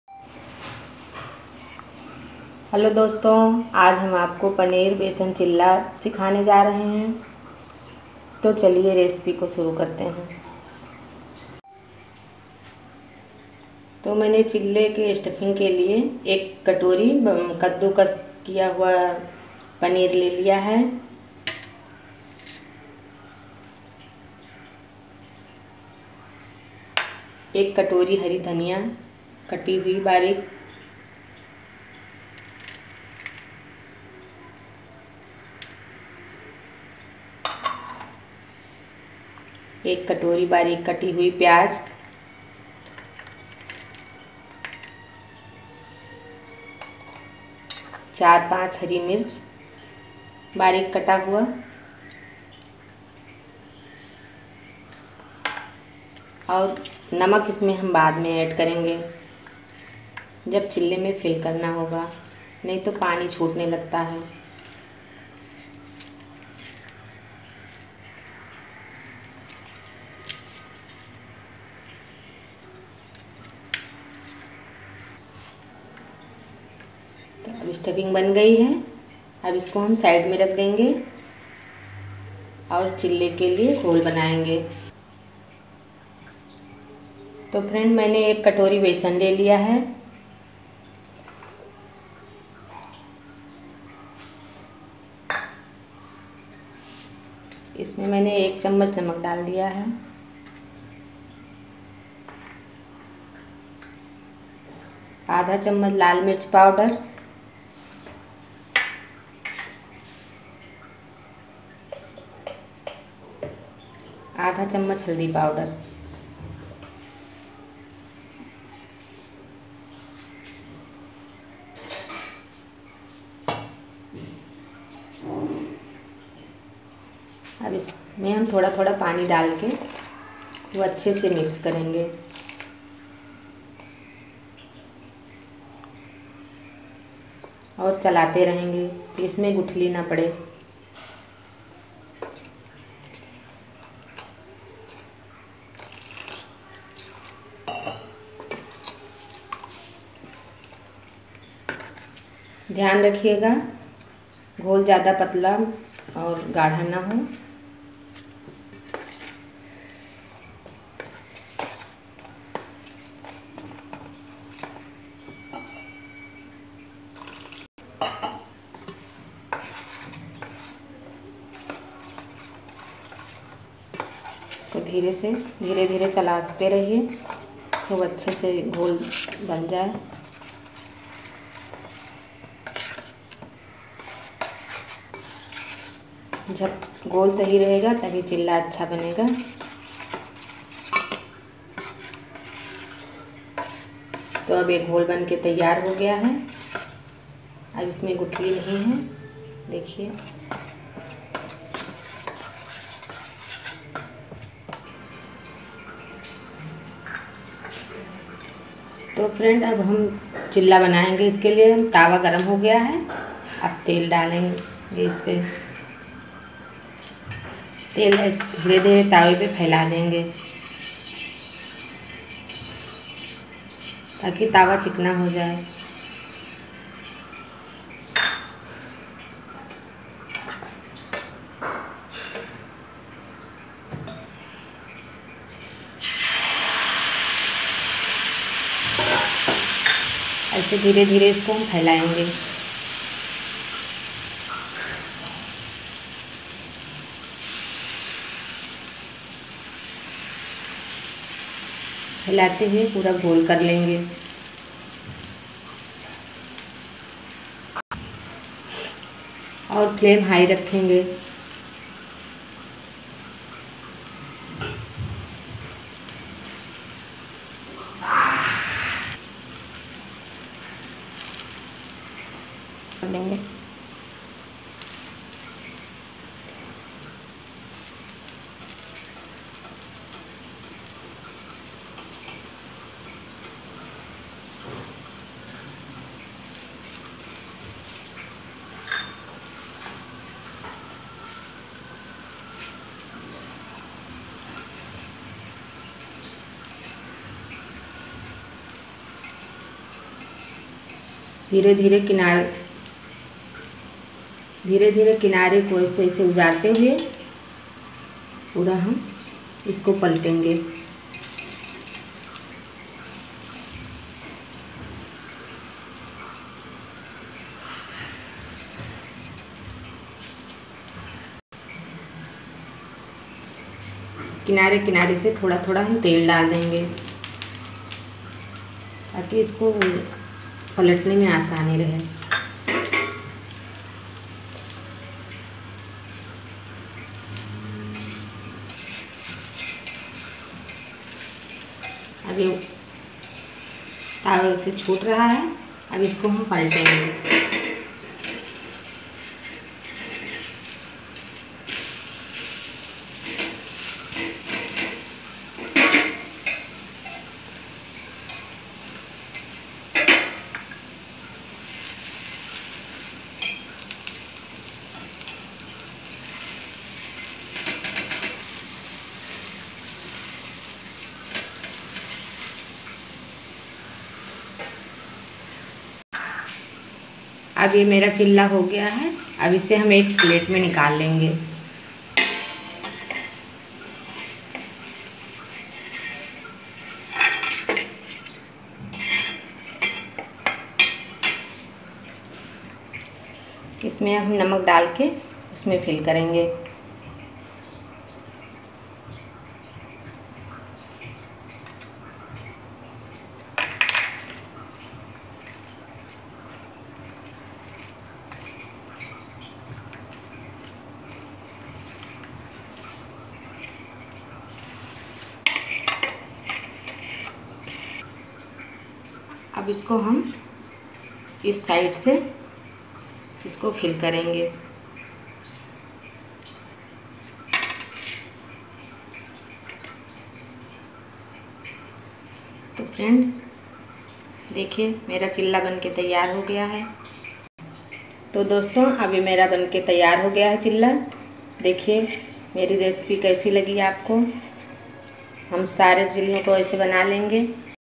बेसन पनीर चिल्ला रेसिपी
Ingredients required
 बेसन एक कटोरी
 हल्दी पाउडर आधा चम्मच
 लाल मिर्च पाउडर आधा चम्मच
 गरम मसाला आधा चम्मच
 पनीर कद्दूकस किया हुआ एक कटोरी
 एक कटोरी प्याज बारीक कटा हुआ
 एक कटोरी बारीक कटी हुई धनिया की पत्ती
 बारीक कटी हुई मिर्च 5-6
 नमक स्वाद अनुसार
 तेल